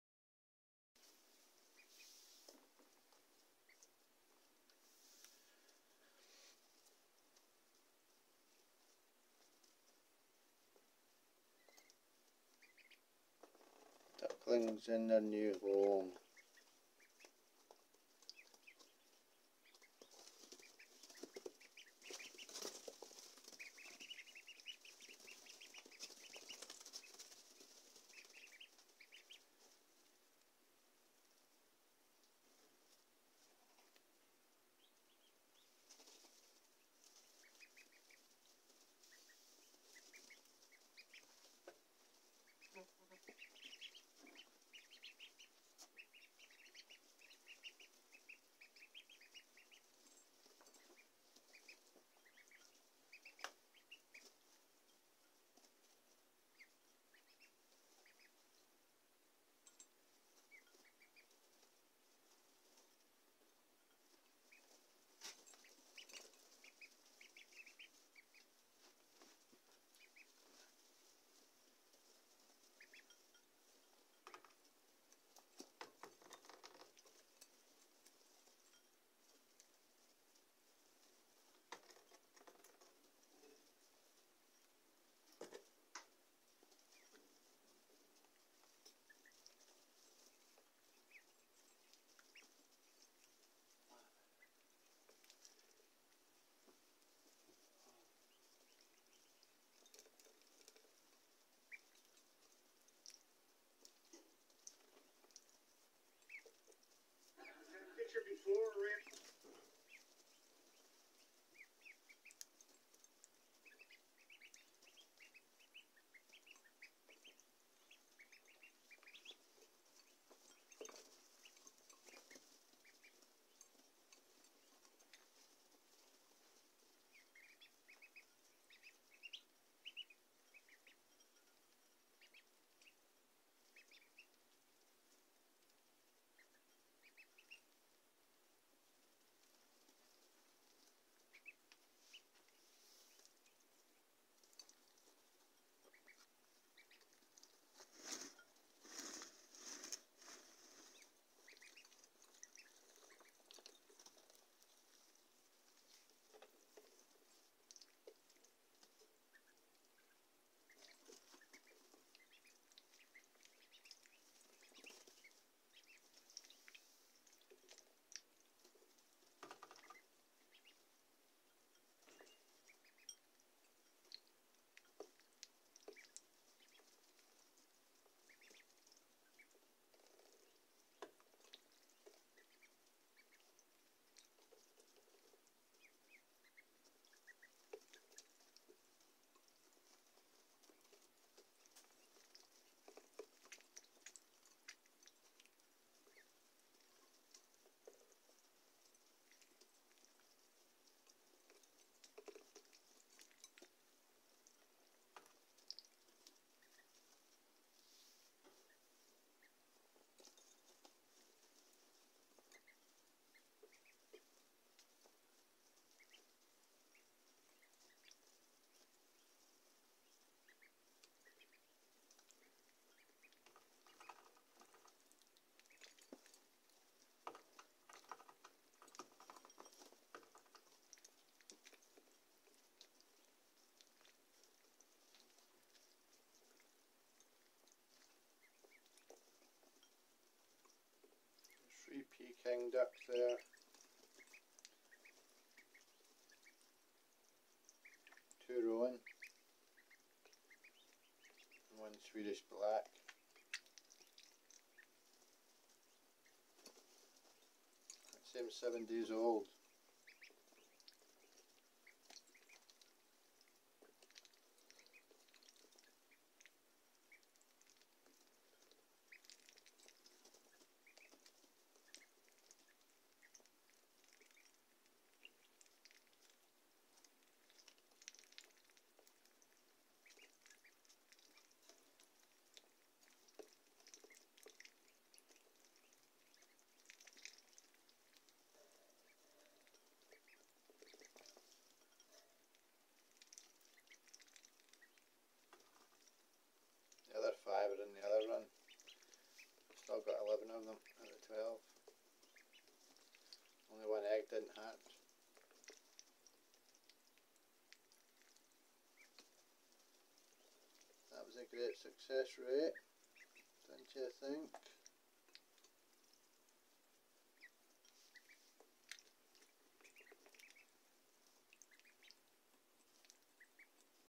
1 WEEK OLD DUCKLINGS, DOING GREAT AND IN THEIR NEW BIGGER HOME
1 WEEK OLD DUCKLINGS, DOING GREAT AND IN THEIR NEW BIGGER HOME

ITS FREE, THANKU